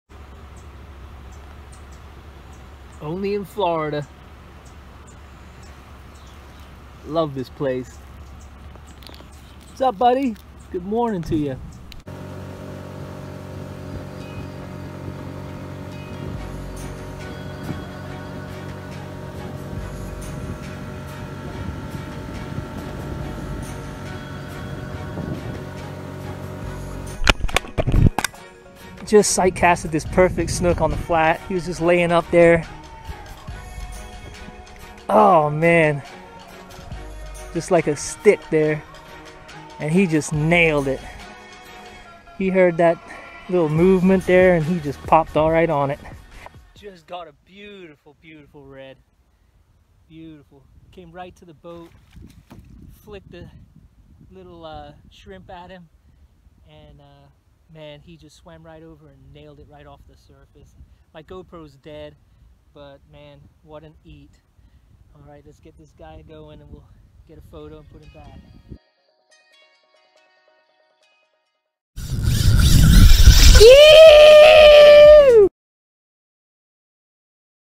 Everglades National Park Red Fish | Fly and Fish Tv
Quick Clip of one of the trips on the Geenoe up in the shallow water of Everglades National Park hunting Snook and Redfish.

SOUNDTRACK